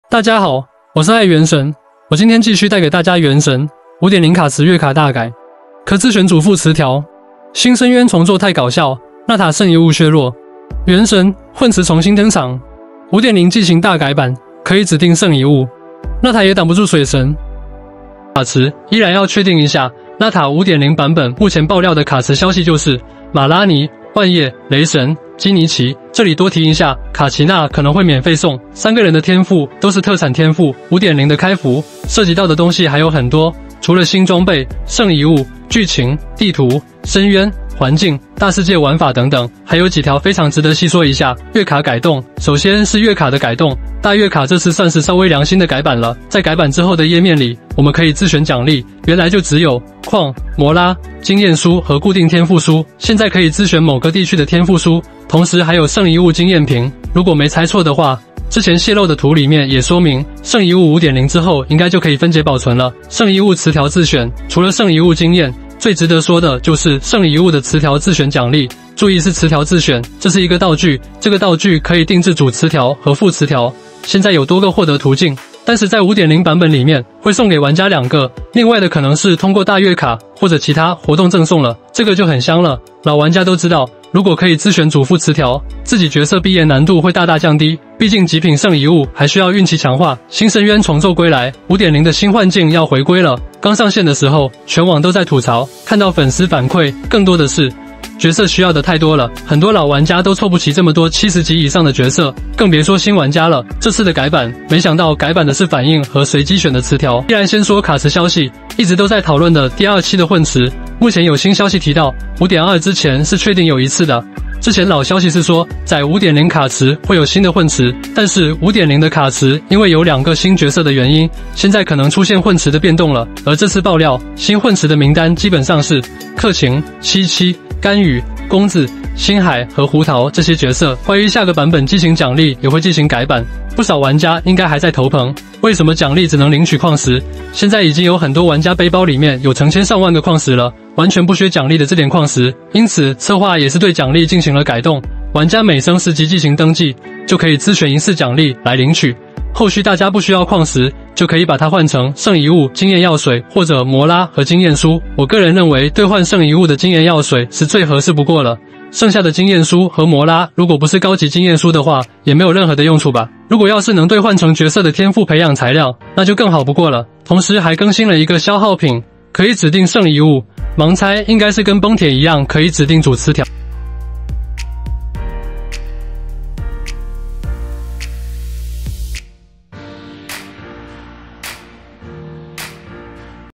原神：5.0卡池月卡大改！可自選主副詞條。新深淵重做太搞笑，納塔聖遺物削弱。原神：混池重新登場。 5.0紀行大改版，可以指定聖遺物？納塔也擋不住水神
Genshin Impact: 5.0 card pool monthly card major changes! You can choose the main and sub-items. The new Abyss rework is too funny, Nata's relics are weakened. Genshin Impact: Mixed pool reappears. 5.0 Chronicle major revision, can you specify the relics? Nata can't stop the water god